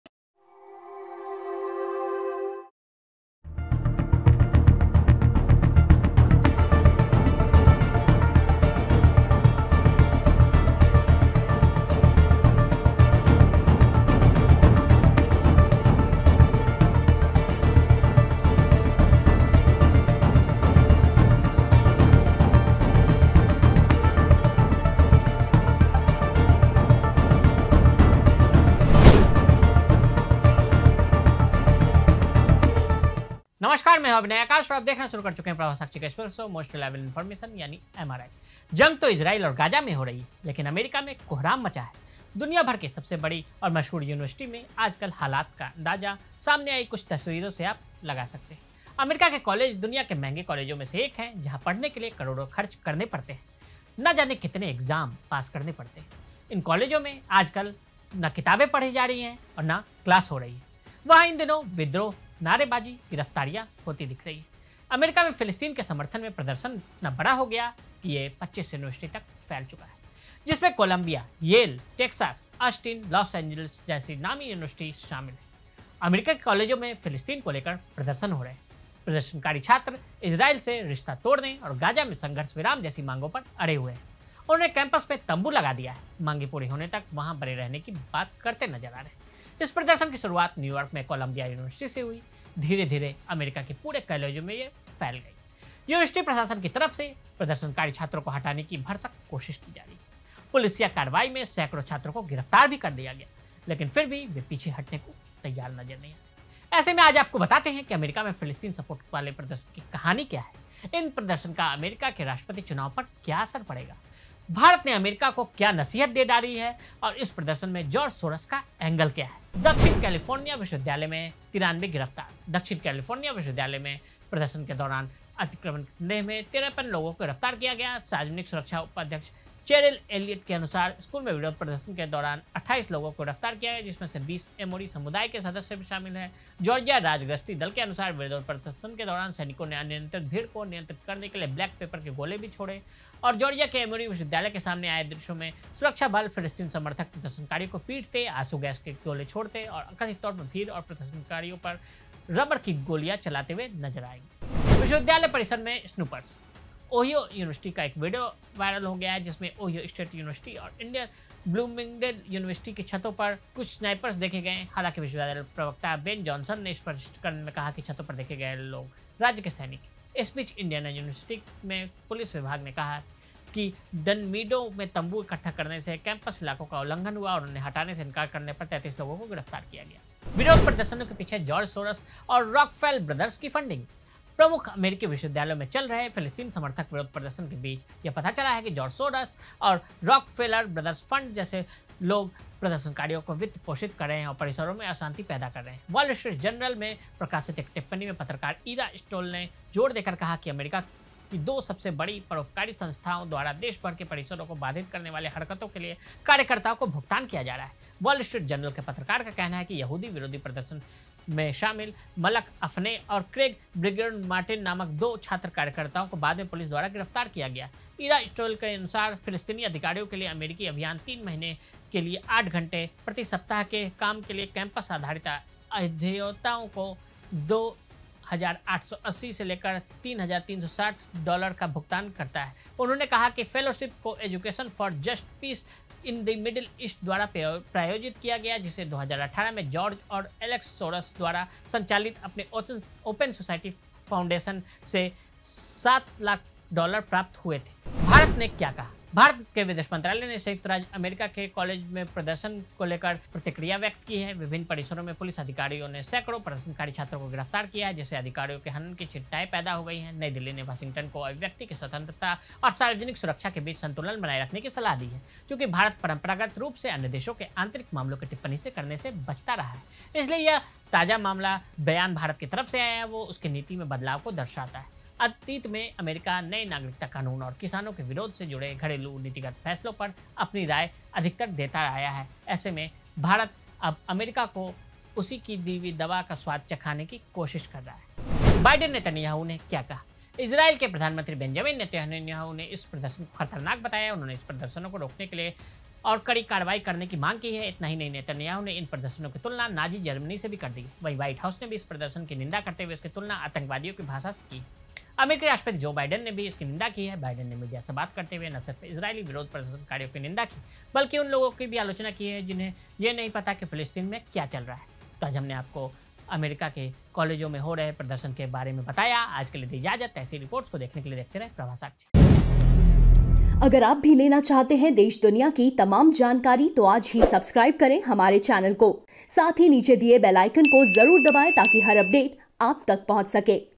American University Protest|अमेरिका में छात्रों की सबसे बड़ी बगवात की वजह? |MRI Prabhasakshi Special
आज आपको बताते हैं कि अमेरिका में फिलीस्तीन सपोर्ट वाले प्रदर्शन की कहानी क्या है? इन प्रदर्शनों का अमेरिका के राष्ट्रपति चुनाव पर क्या असर पड़ सकता है। भारत ने अमेरिका को क्या नसीहत दी है और इस प्रदर्शन में जॉर्ज सोरेस का एंगल क्या है।